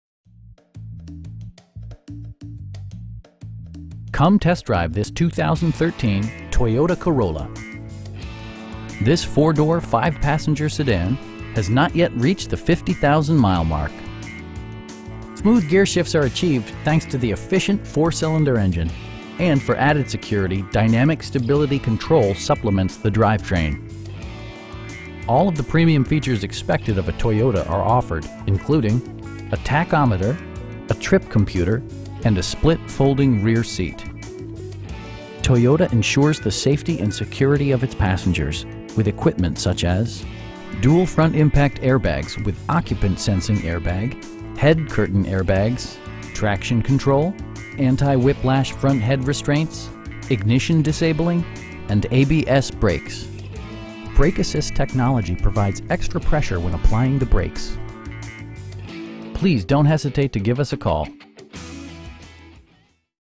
2013 Toyota Corolla LE in Fredericksburg, VA 22408
Rosner Toyota Fredericksburg
New: 3507 Jefferson Davis Highway
Used: 3427 Jefferson Davis Highway

Come test drive this 2013 Toyota Corolla. This 4 door, 5 passenger sedan has not yet reached the 50,000 mile mark! Smooth gearshifts are achieved thanks to the efficient 4 cylinder engine, and for added security, dynamic Stability Control supplements the drivetrain. All of the premium features expected of a Toyota are offered, including: a tachometer, a trip computer, front bucket seats, air conditioning, telescoping steering wheel, power door mirrors, and a split folding rear seat. Toyota ensures the safety and security of its passengers with equipment such as: dual front impact airbags with occupant sensing airbag, head curtain airbags, traction control, anti-whiplash front head restraints, ignition disabling, and ABS brakes. Brake assist technology provides extra pressure when applying the brakes. Our aim is to provide our customers with the best prices and service at all times. Please don't hesitate to give us a call.